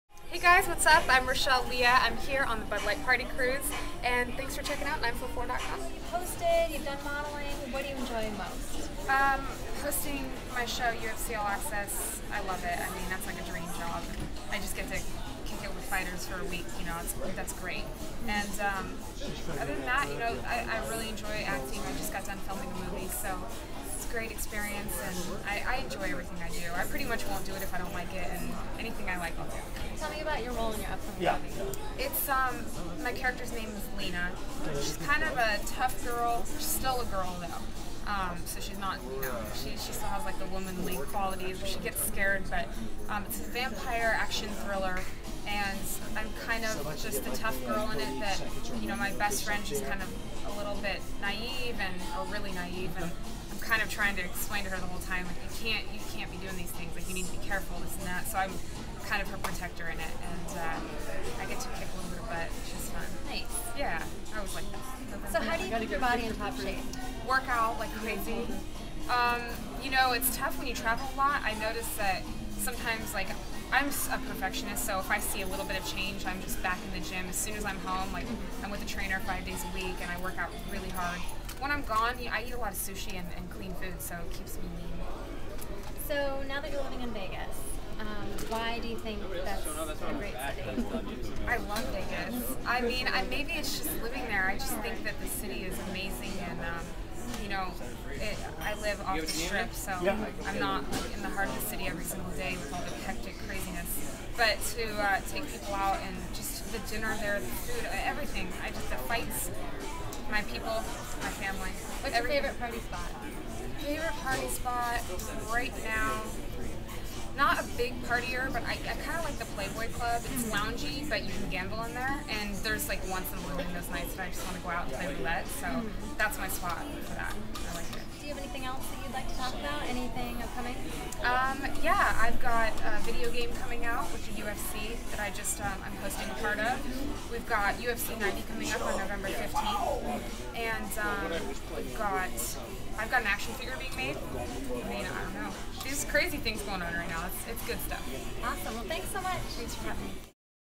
944 Interview with Rachelle Leah, Playboy Cover Model
944 interviews Rachelle Leah, the November 2008 Playboy Cover Model, aboard the Bud Light Party Cruise. The UFC, her new movie, how she stays in shape, what she loves about Las Vegas, and more! Check out 944.com for more photos of Rachelle.